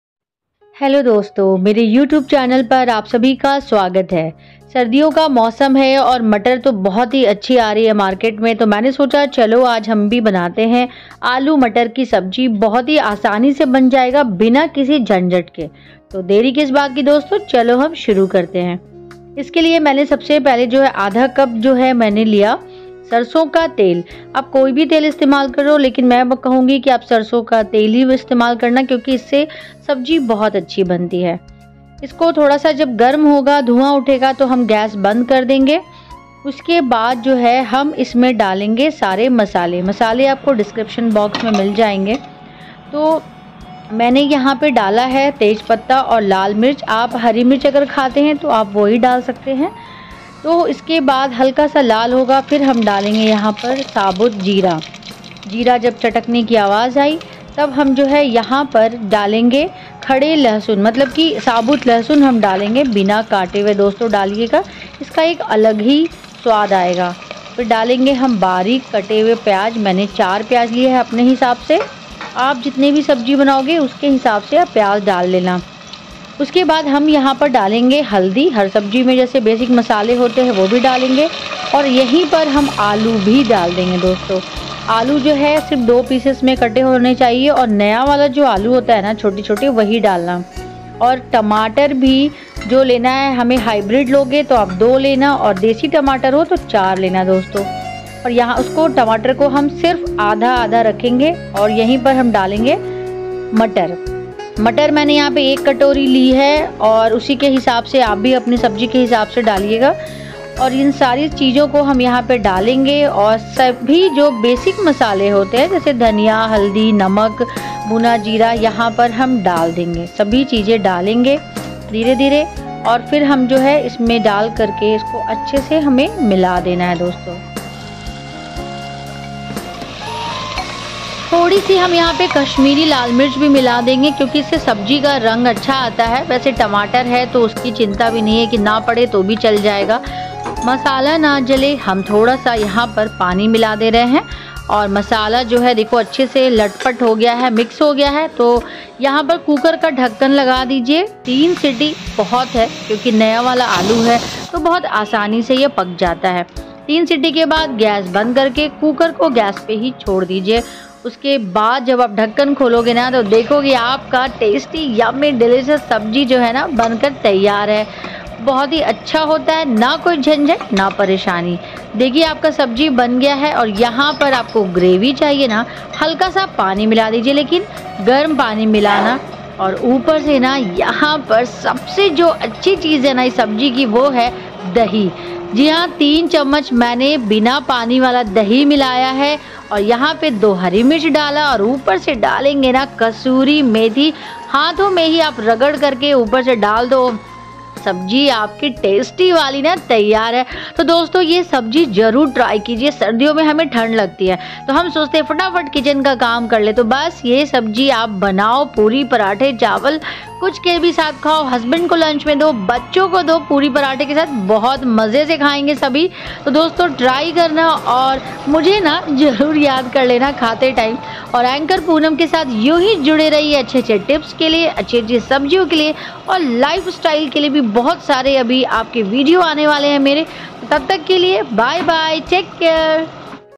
Aloo Matar Recipe😋 | नये वाले छोटे आलू और मटर की तरीदार सब्जी ☺ | Tasty Aloo Curry
सर्दियों में आलू मटर की सब्जी जरूर बनाएं क्योंकि मटर खाने से कोलेस्ट्रोल कंट्रोल रहता है। मटर में नियासिन नाम का तत्व भरपूर होता है। मटर में पर्याप्त मात्रा में आयरन, जिंक, मैगनीज और कॉपर मौजूद होता है. इतना ही नहीं इसमें एंटी-ऑक्सीडेंट गुण पाया जाता है, जो इम्यूनिटी को बढ़ाने में मददगार है. हरी मटर में फाइबर भी अच्छी मात्रा में पाया जाता है, जो पाचन को बेहतर रखने में मदद कर सकता है.